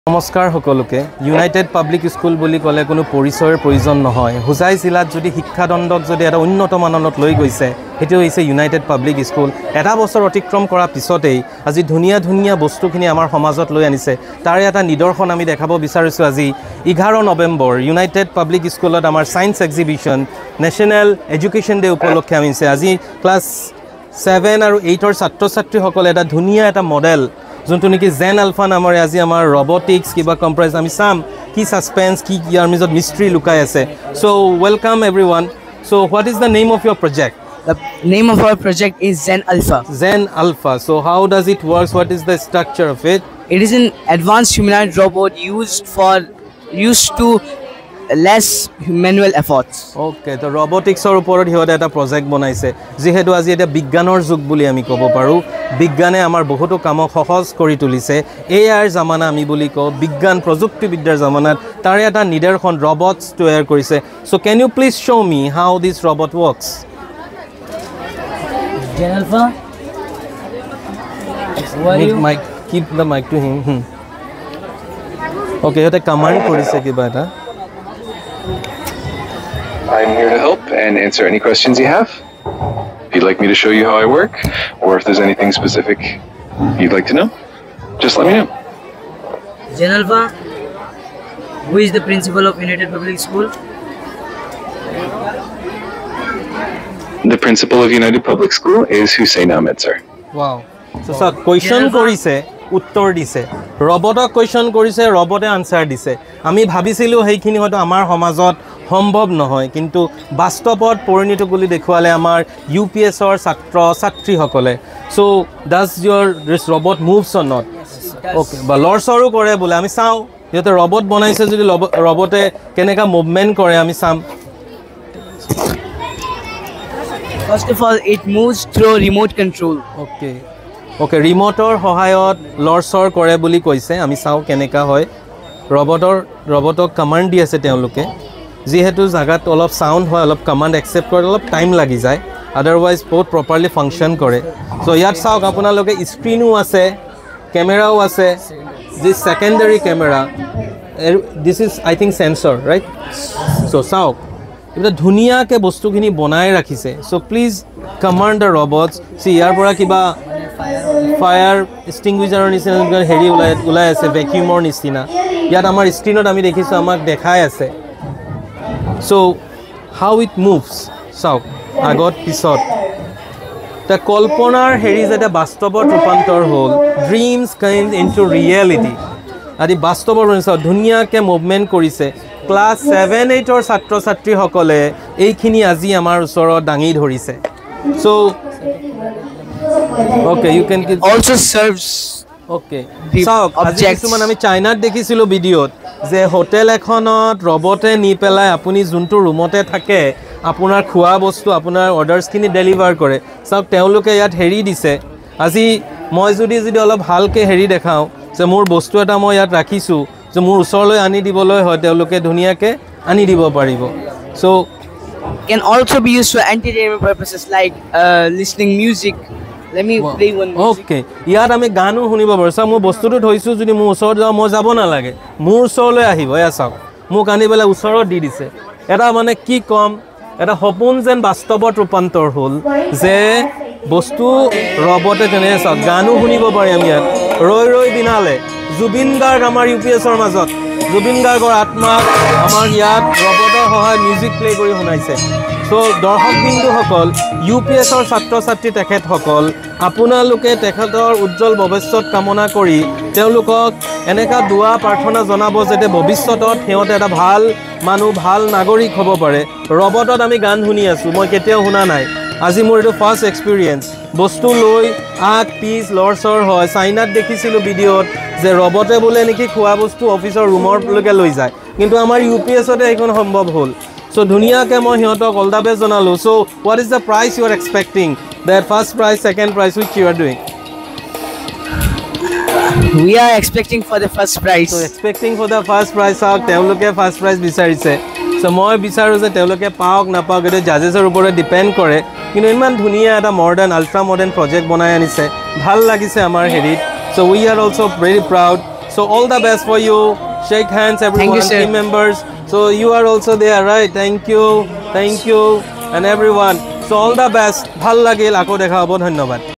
AI Robot
Robotics video displayed at UPS,Hojai science exhibition by the students of class 7and 8
witness the features by watching the video till end. i
It will enhance your inner thoughts to the zenith